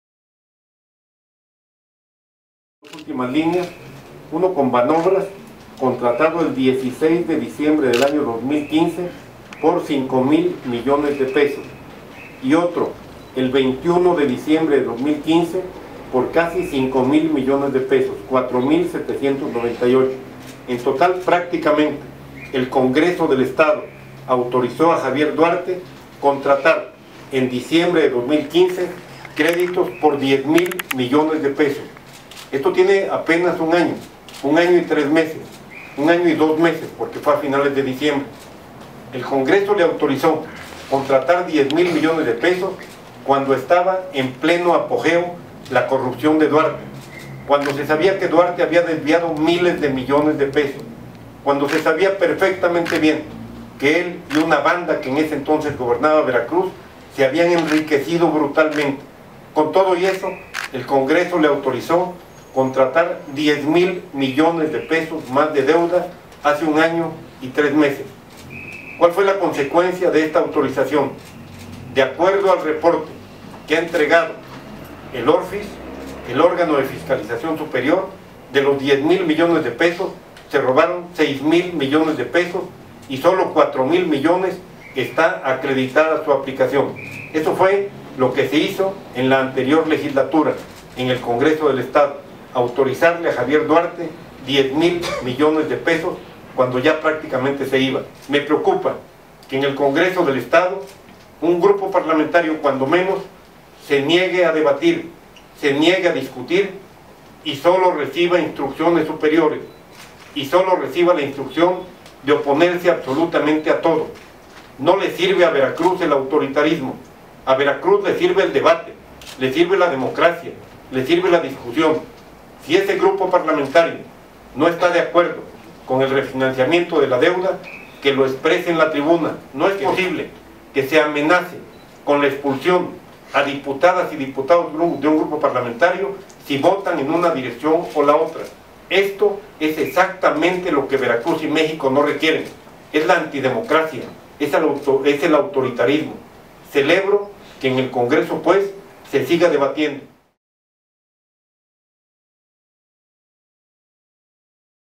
Legislatura pasada aprobó crédito a Duarte, cuando prácticamente se iba: MAYL
Xalapa, Ver.- En conferencia de prensa, el gobernador del estado de Veracruz, Migel Ángel Yunes Linares, denunció que la antigua Legislatura, aprobó 10 mil millones de pesos a Javier Duarte, lamentó que la actual no quiera debatir para la iniciativa de Reestructuración de la Deuda en Veracruz.